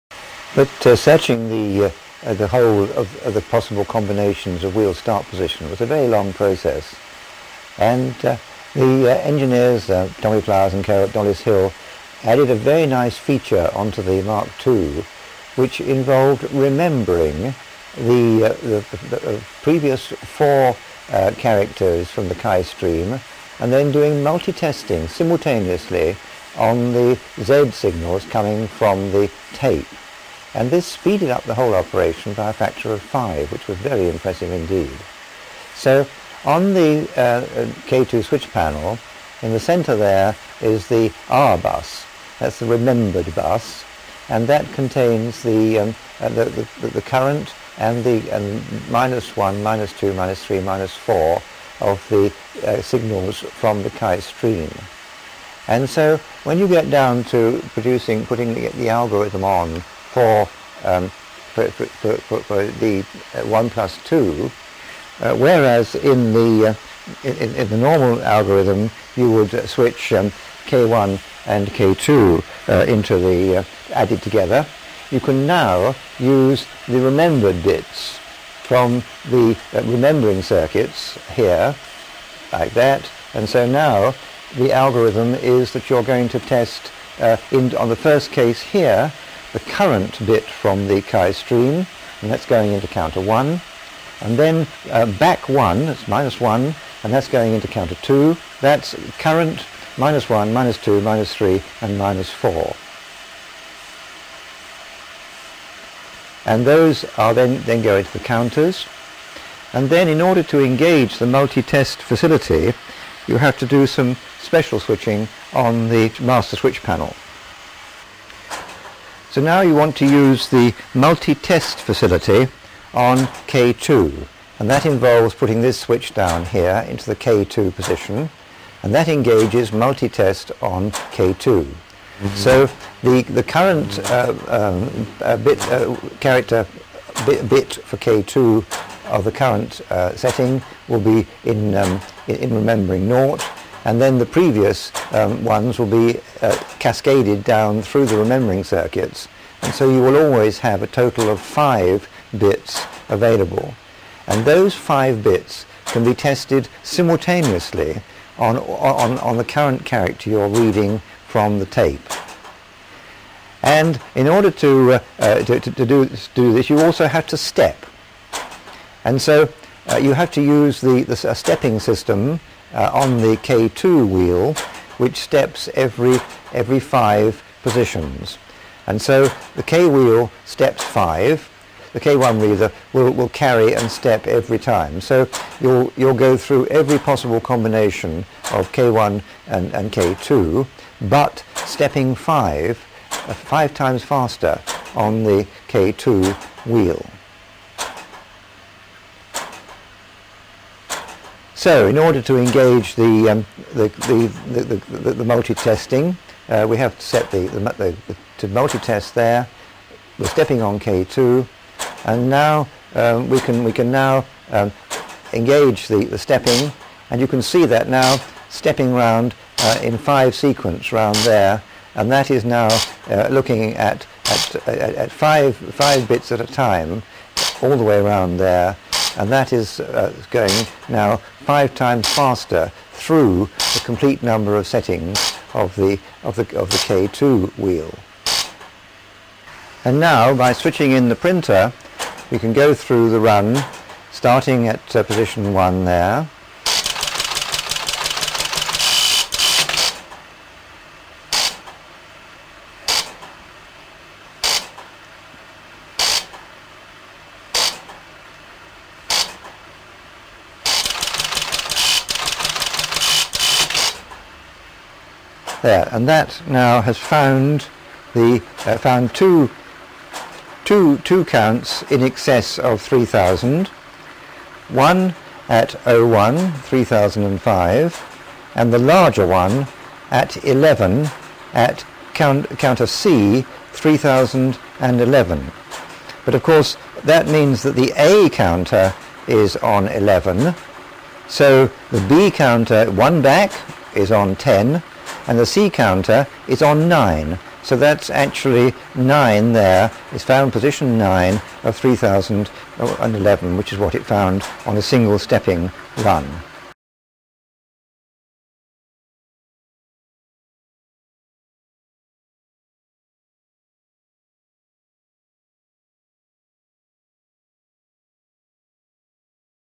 Colossus - The First Electronic Computer - Pt2
Demonstration video of the rebuilt Colossus computer at the Bletchley Park Museum in action.  This machine was designed during WWII to break the German Lorenz cipher.